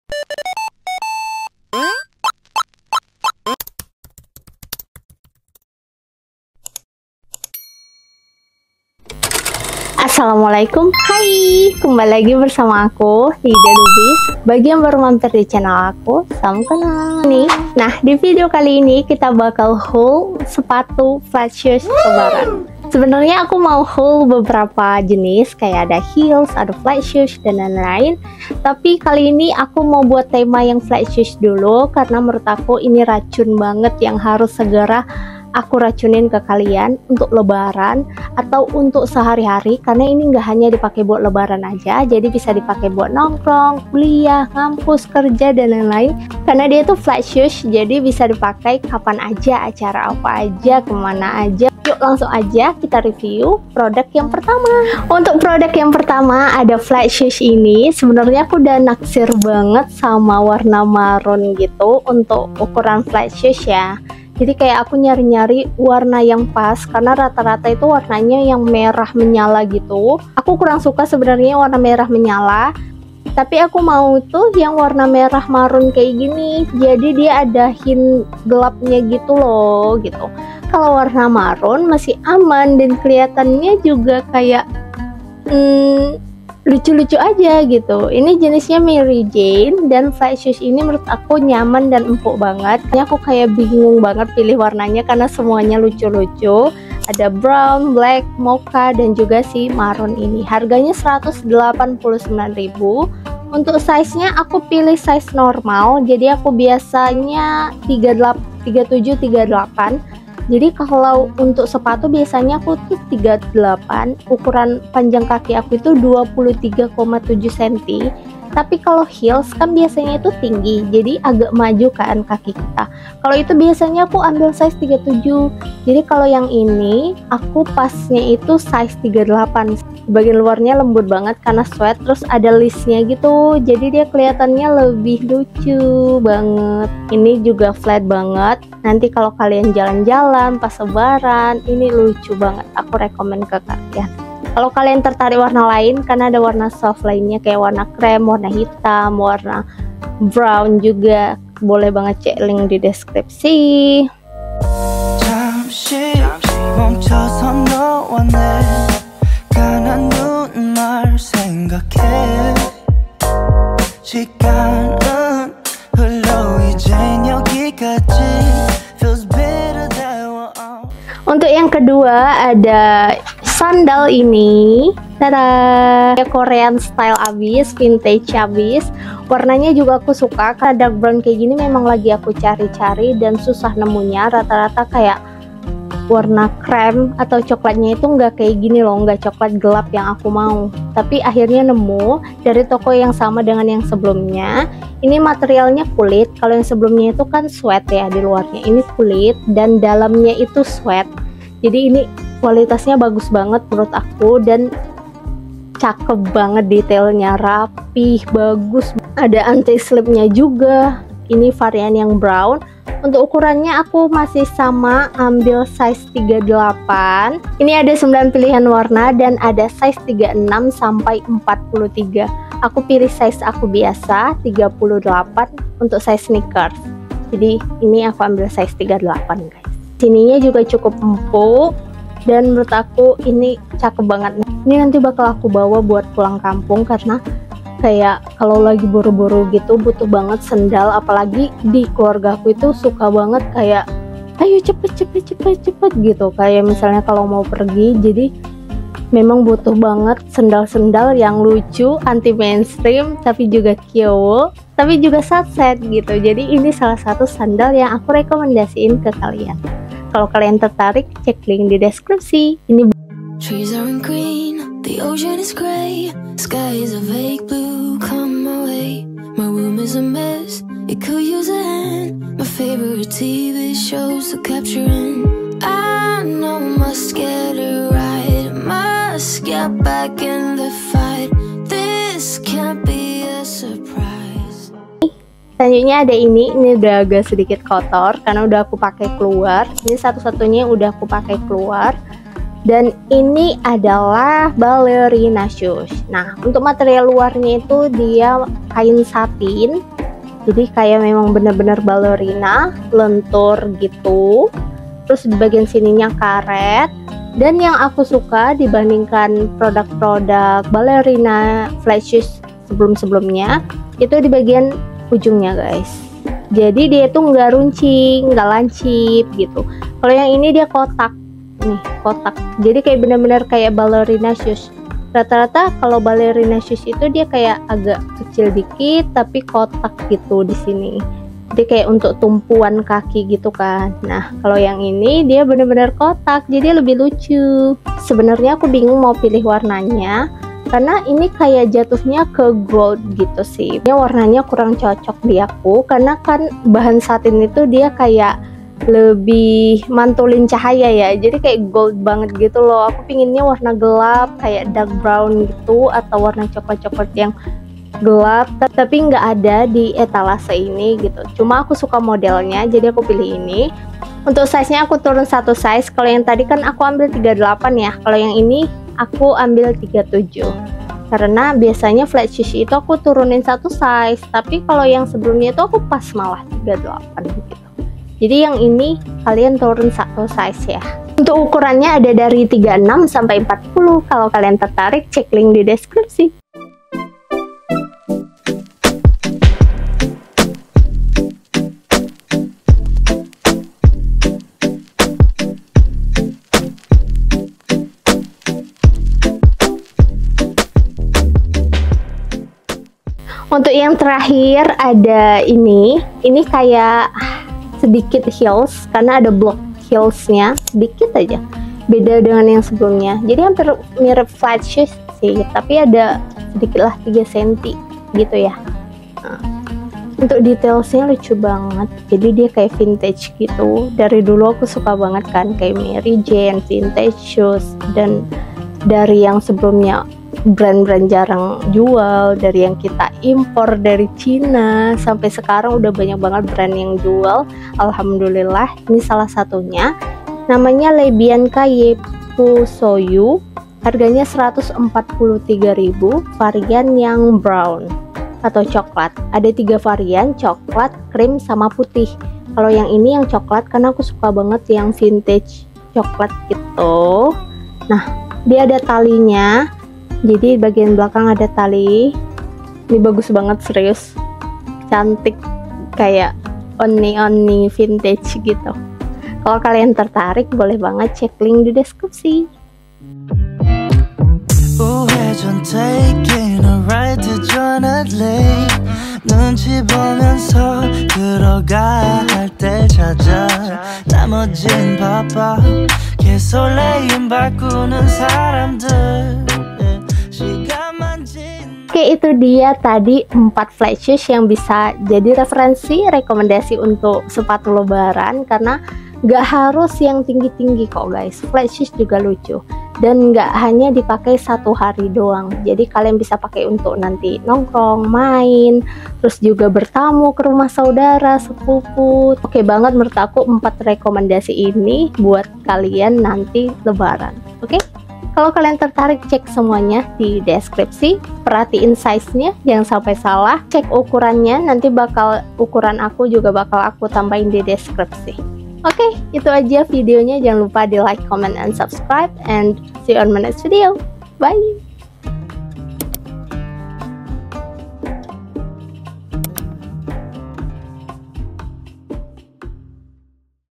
SHOPEE HAUL SEPATU DAN SENDAL LEBARAN KIYOWO!🎀
Hi guys Assalamualaikum !

Video kali ini shopeehaulootdlebaran 2024 part 3 edisi sepatu !!
jika suka dengan video ini, silahkan di like juga ya.

Ukuran kaki lilu:
Panjang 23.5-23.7cm (biasa pake sneakers 37&38(lebih nyaman 38 tapi kalau sandal/heels biasa 37)

🌸Review flatshoes lebaran